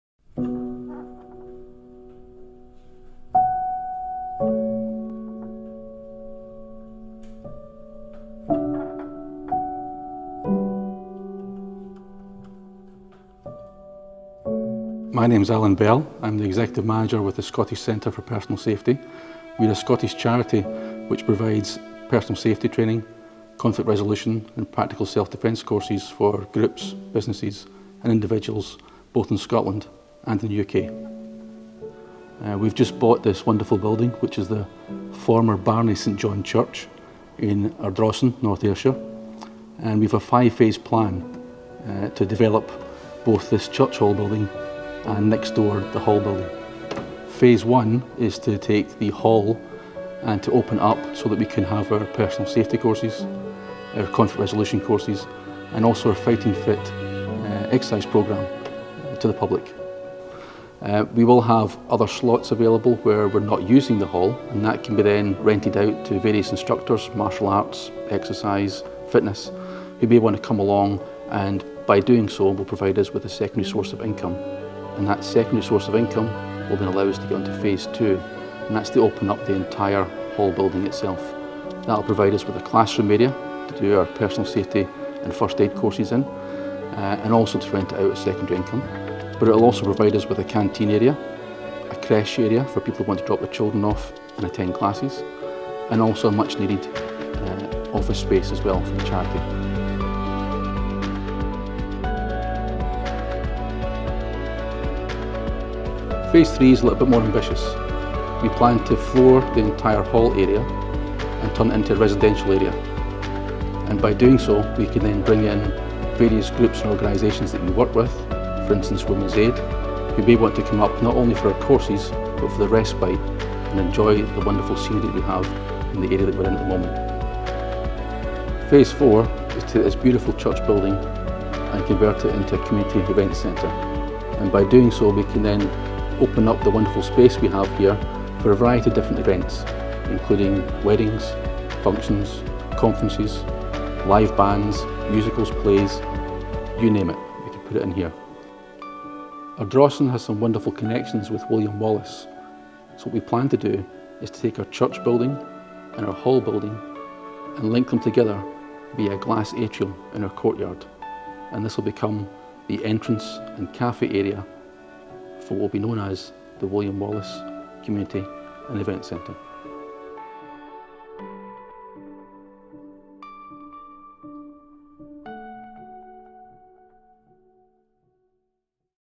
The Scottish Centre for Personal Safety
Alan Bell, Founder and Executive Manager of the Scottish Centre for Personal Safety gives a overview of the charity and outlines the plans for the former St. Barony Church in Ardrossan North Ayrshire.